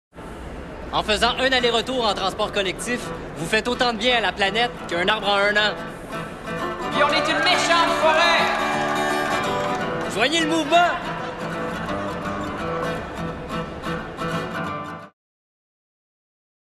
STM - PUB - "Joignez le mouvement"
La STM lance son offensive publicitaire printanière, qui fait une fois de plus valoir son côté vert.

Conçue par son agence Sid Lee sous le nouveau thème «Chaque passager compte pour la planète», la campagne comprend de l'affichage sauvage, des pubs dans les journaux, en ligne, dans le métro, mais aussi des pubs télé. Où l'iconographie développée pour la nouvelle identité visuelle de la STM, dévoilée en 2009, prend vie.

Cette pub met l'accent sur les bénéfices écologiques en citant une statistique : un aller-retour en transport collectif fait autant de bien à la planète qu'un arbre en un an. Dans un escalier, des usagers tiennent alors chacun un arbre en carton, symbolisant ainsi le résultat collectif. Sur son site, la STM explique comment elle parvient à ce calcul.

L'offensive s'adresse surtout aux usagers occasionnels de la STM, et à ceux qui ne l'utilisent pas du tout.

«Cette campagne vise à faire changer nos habitudes de vie puisque chaque geste compte pour la planète. En mettant en place une campagne qui parle du gros bon sens de façon ludique et en utilisant un média de masse, la STM et Sid Lee souhaitent encourager les changements de comportement. Ainsi, lorsque viendra le temps de démarrer leur voiture, les Montréalais n'auront d'autre choix que de penser au transport collectif comme une option bien sensée! La tonalité de la campagne télé est aussi la suite logique de l'image de marque qui a été lancée en 2009. Nous avons créé une image et une voix en parfaite harmonie, c'est ça la force d'un bon branding, selon nous», souligne dans un communiqué Hélène Godin, directrice de création.